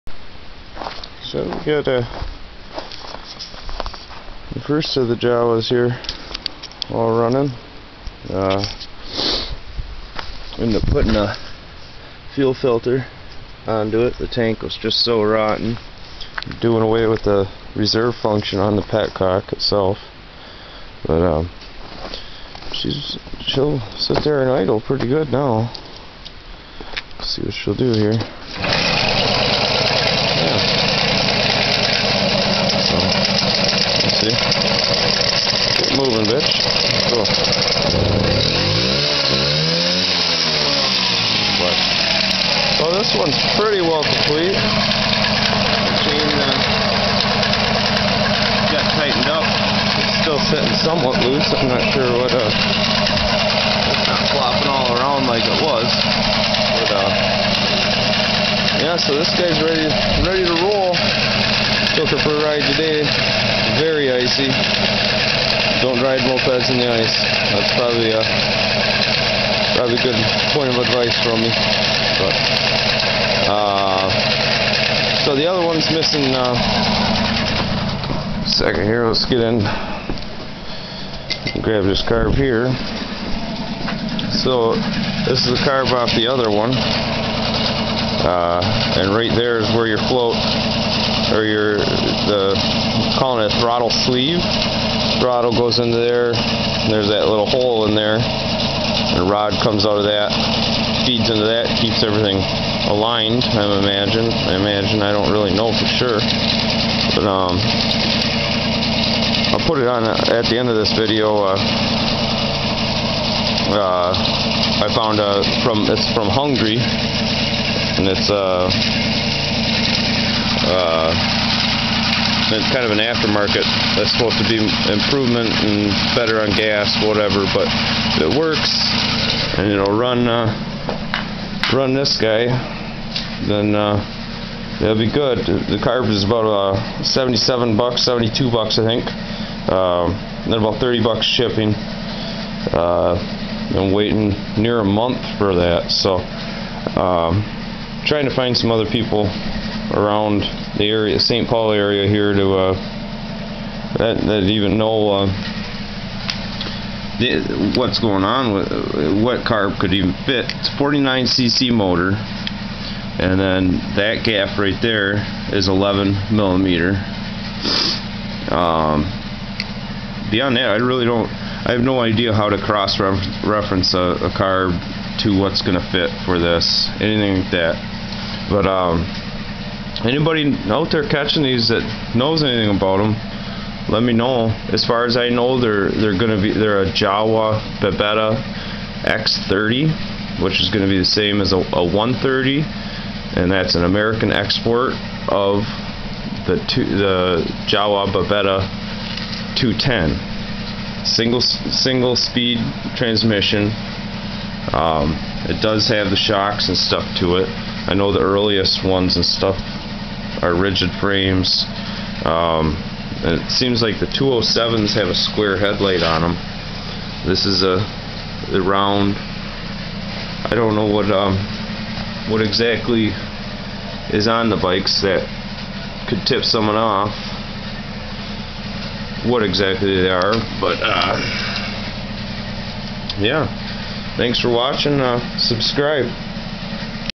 black jawa ready to go
1979 49cc jawa babetta x-30. a very varnished gas tank some, pluged petcock  and a dirty carb later shes ready to go.